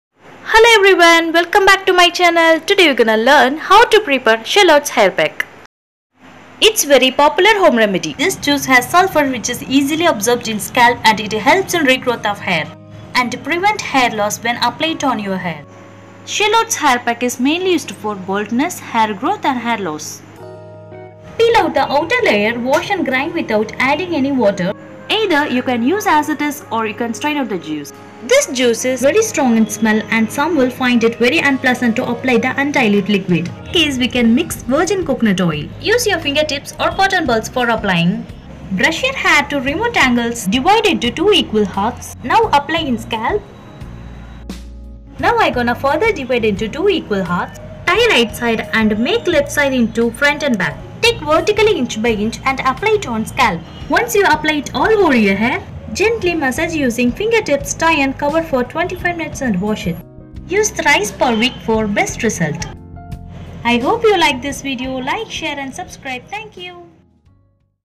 Shallots hair pack
Shallots hair pack mainly used for hair loss baldness hair growth and skin infection this pack it helps to Regrowth and prevent hair loss.